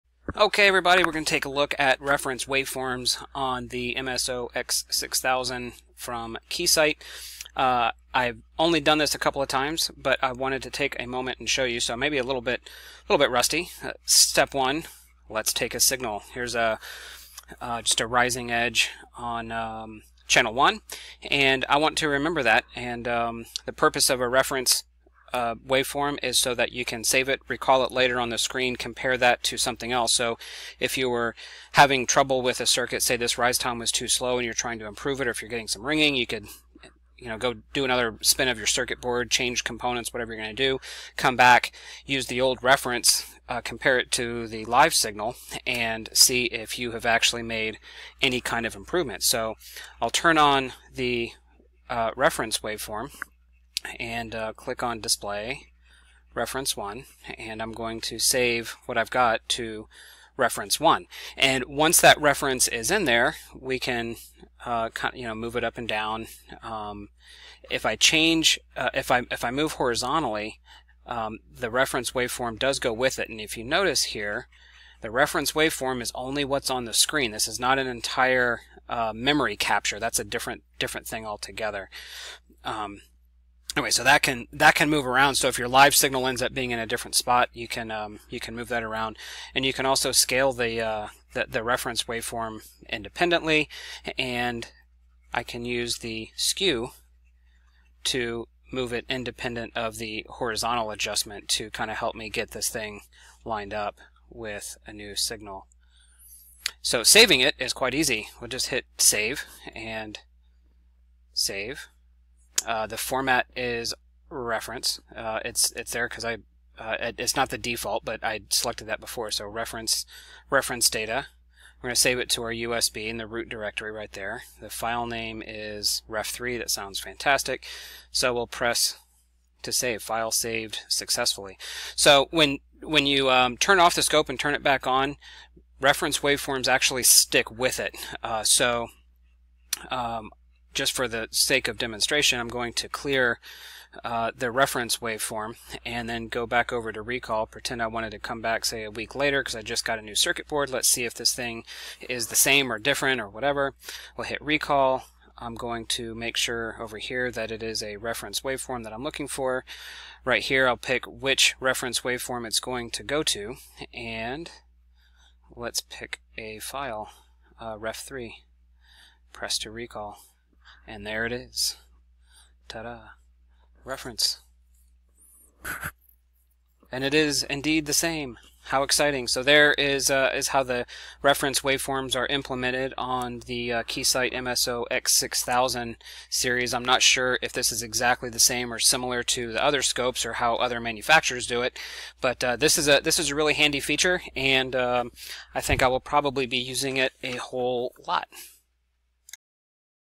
Keysight MSOX6000 REF Waveforms
The Keysight MSOX6000 scope provides a handy tool to save up to four waveforms to be used as references in the future. They are easy to save and recall from a USB drive.

As your circuits are designed, you can save various trouble spots that you need to change in some way. This makes it easy to come back to a measurement after you made some changes and compare the old and the new.  Really handy.